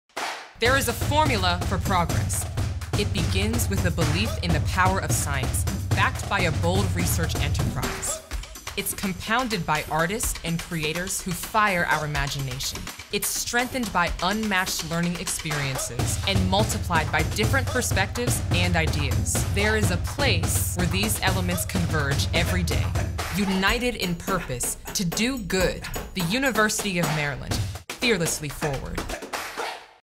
Fearlessly Forward | The Future Is Here
We have the formula for progress. Scholars, researchers, innovators and creators converge at the University of Maryland to shake up the status quo and be a force for good. Together, we lead Fearlessly Forward.